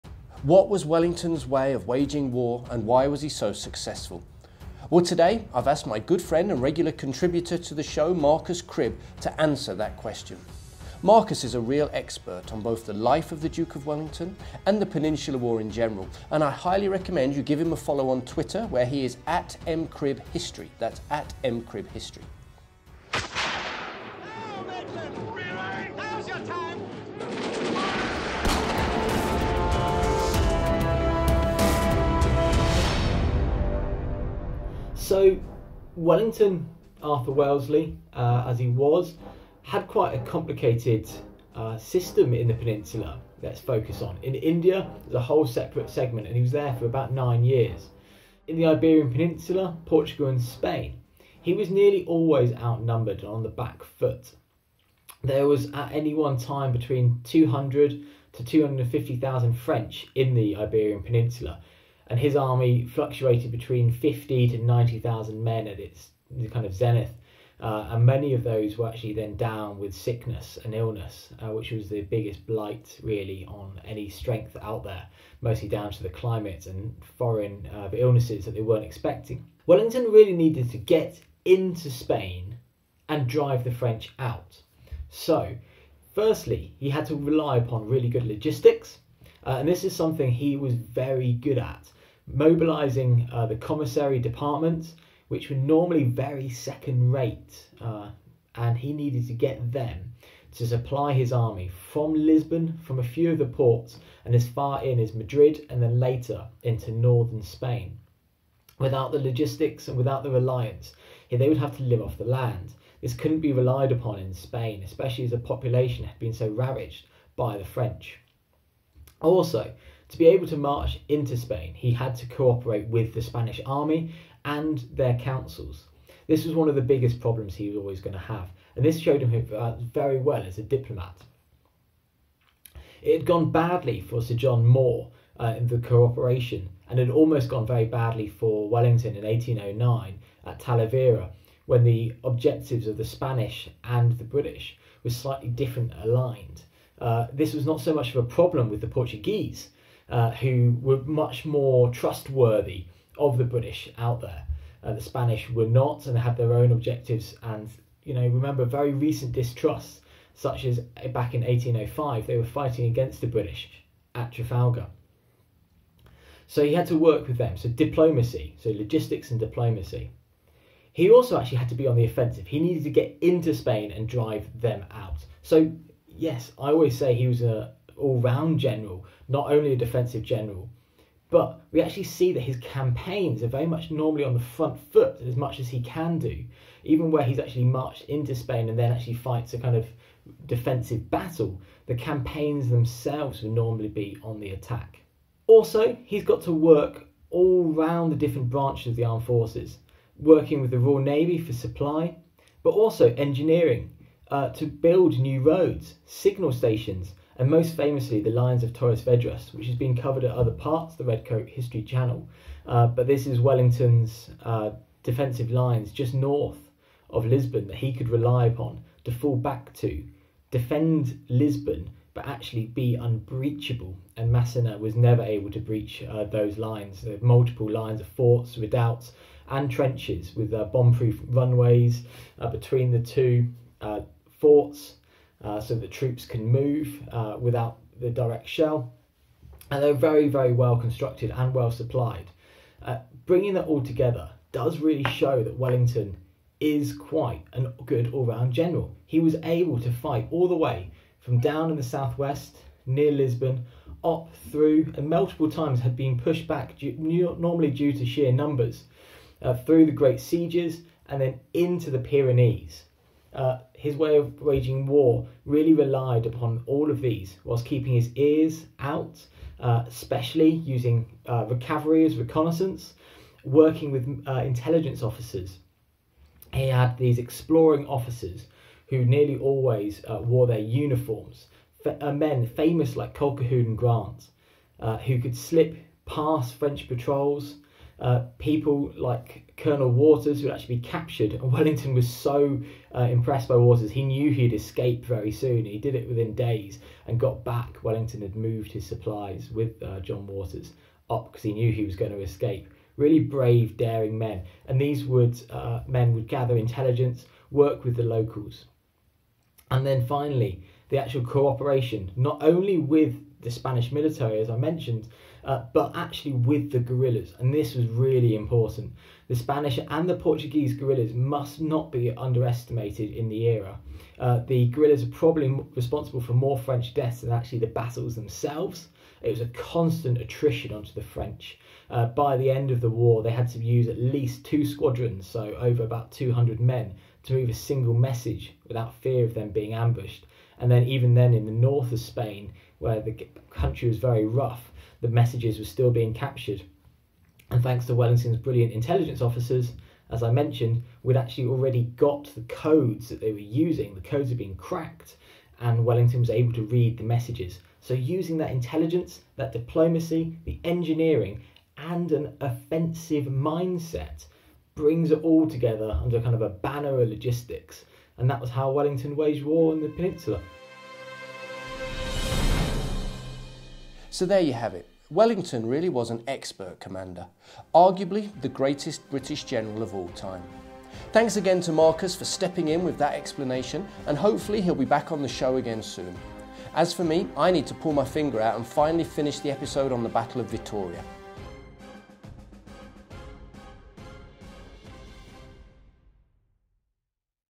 Why was the Duke of Wellington so successful during the Peninsular War?
The Duke of Wellington is probably Britain's greatest ever general. But what were the factors that made him so effective during the Peninsular War against the French?

In this "Five Minute Friday" my good friend Marcus Cribb (@mcribbhistory on Twitter) breaks down the Duke's success.

If you are on Instagram and like looking at photos of the world's battlefields then check me out